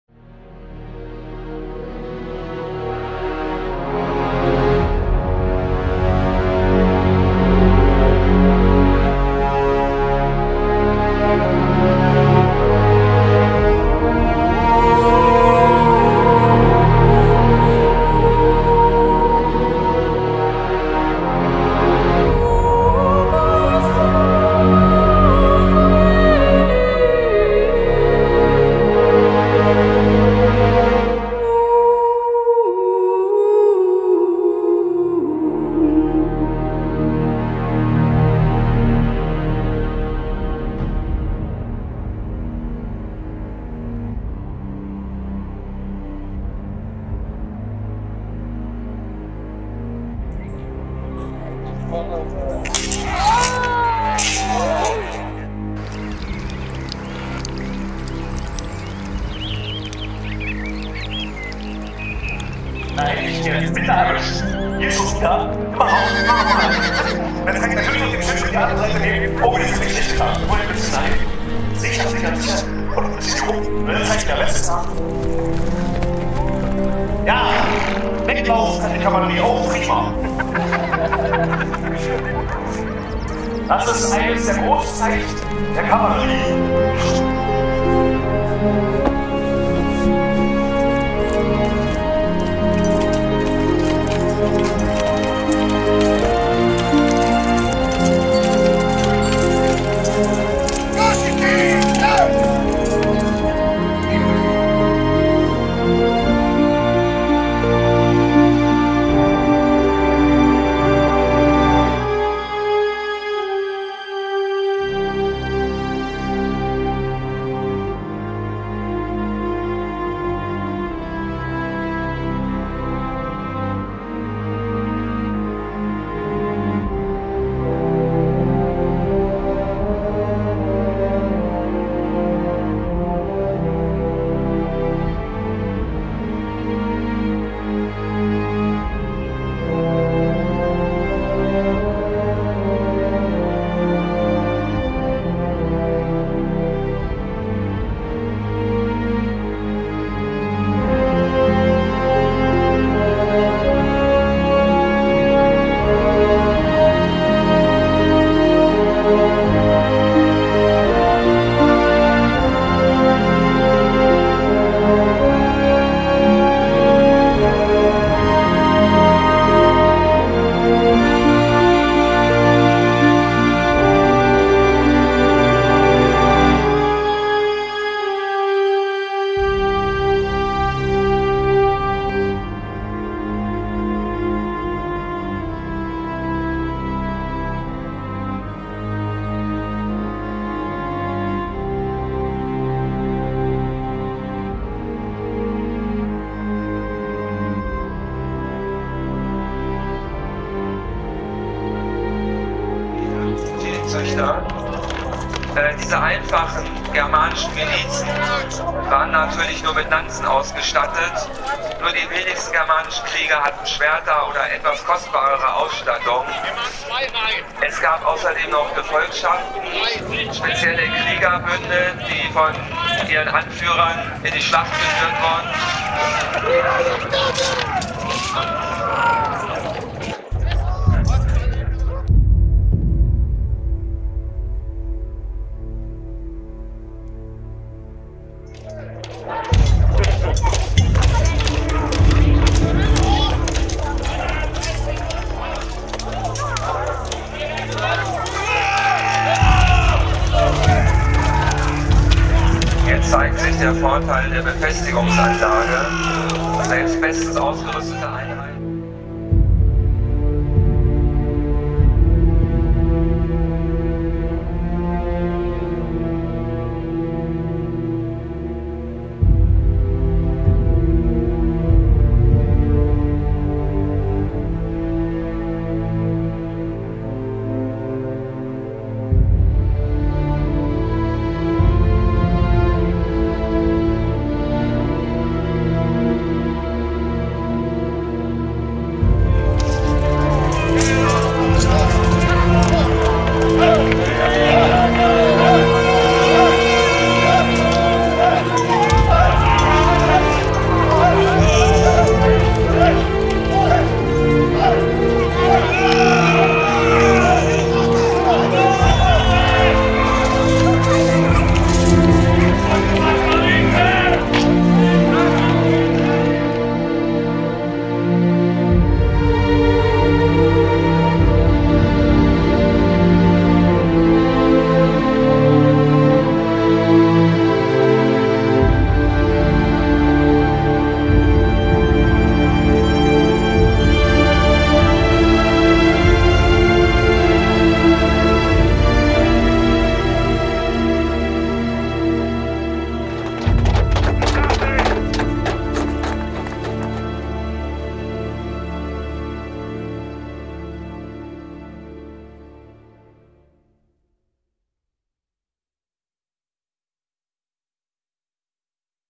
Die Varusschlacht ,Teutoburger Wald
An impression of the reenactment of the the varus battle ("varusschlacht")near Kalkriese,
held on the same ground where the romans were ambushed.